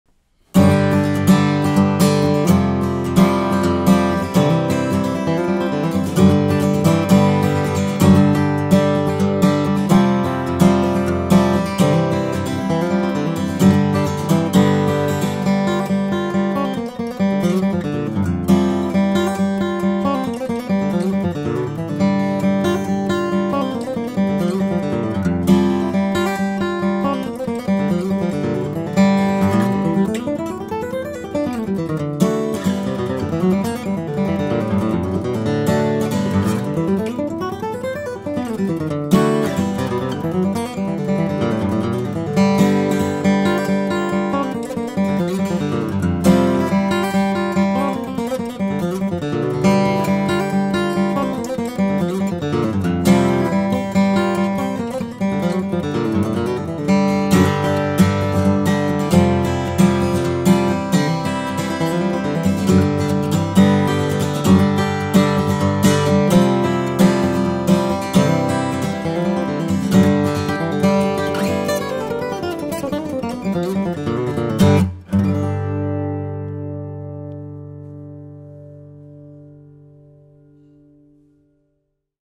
Henderson D-42, Brazilian Rosewood | Carl Miner
For those who have immersed themselves in the world of pre-war inspired guitars, luthier Wayne Henderson needs no introduction. Having built guitars for some of the most demanding and respected players in the world, his name has been carried to the top of the scene. But really, his guitars speak for themselves. Meticulously handmade with many of the materials being self-forged, this Brazilian rosewood D-42 is a testament to what doing things slow, and right, can accomplish. The powerful Brazilian bass will help drive any rhythm section, and the treble notes sing out cleanly and clearly, giving the player melodies and solos that pop. Replete with fine appointments, this D-42 is finished off with abalone from body to headstock, ivoroid binding, and snowflake and diamond inlays. Wayne has very little competition when it comes to true-blue guitar tone, and they don't show up very often on the market.